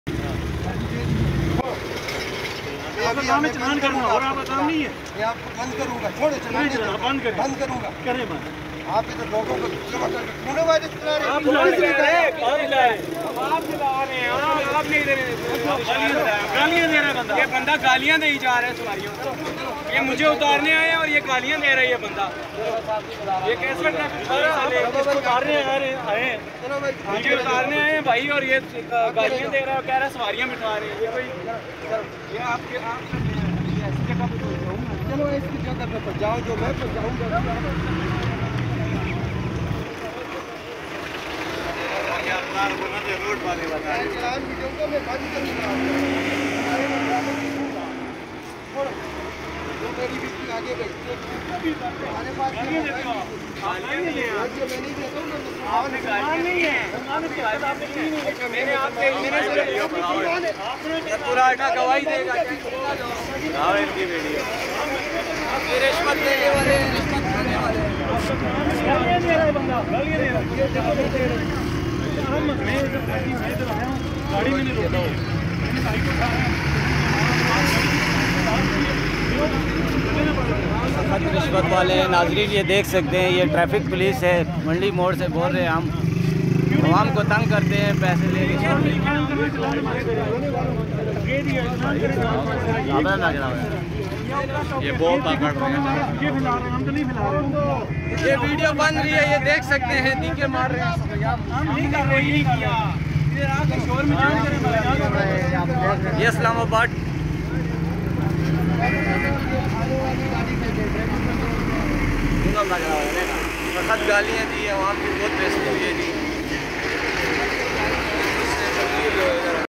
Islamabad Traffic Police | Misbehavior | During Lockdown in Islamabad
Islamabad Traffic Police Misbehavior With Civilian during lockdown in Islamabad Pakistan